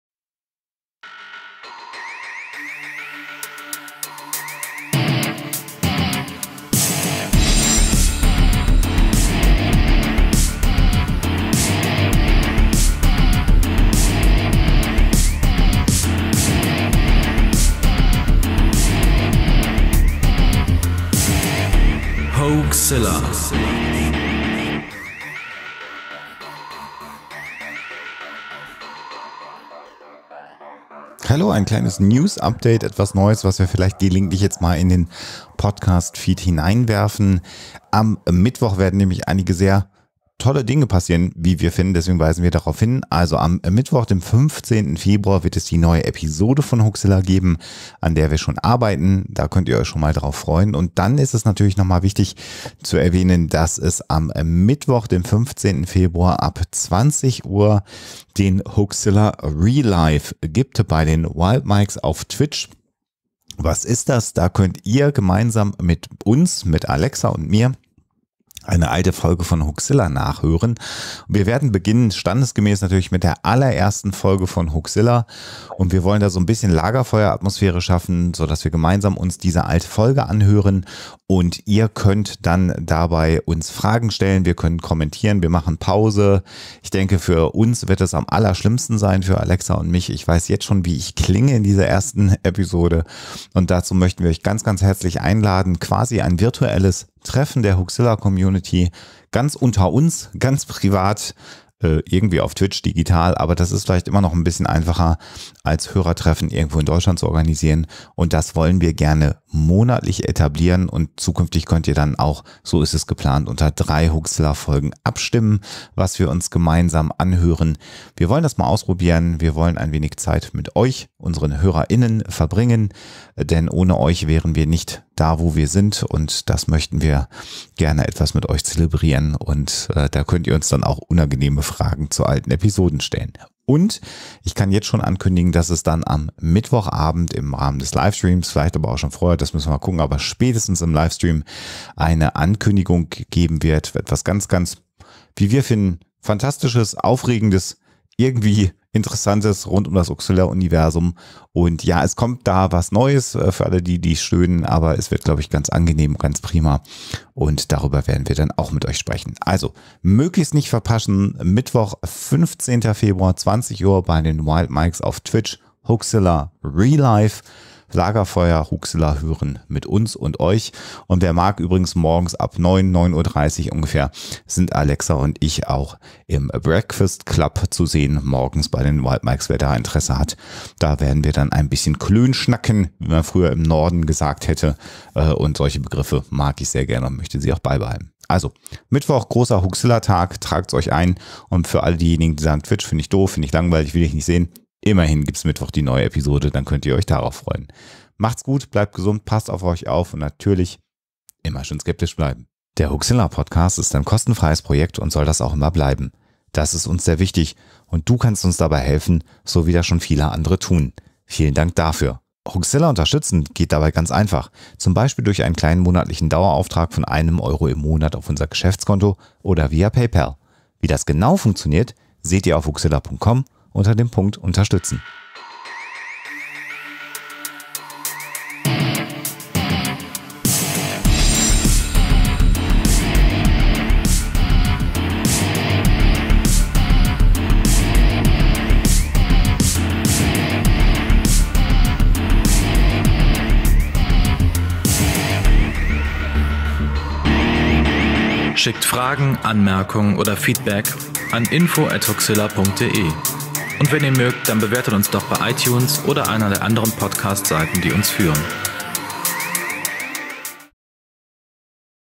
Hoaxilla Newsflash #001
Es gibt einige Ankündigungen für Mittwoch, den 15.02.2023.
Ab 20:00 Uhr wird es wieder den HOAXILLA Re:Live geben. Dort könnt ihr gemeinsam mit uns live alte HOAXILLA Folgen hören und darüber diskutieren.
Beginnen wollen wir mit Folge #01 – Cokelore.
Zudem sind wir am Mittwoch ab ca. 09:30 Uhr beim Breakfastclub der WildMics zu sehen.
Und es wird eine weitere Überraschung am Mittwoch geben – dabei sein lohnt sich also!

Unterstütze unsere Arbeit. Mehr hier erfahren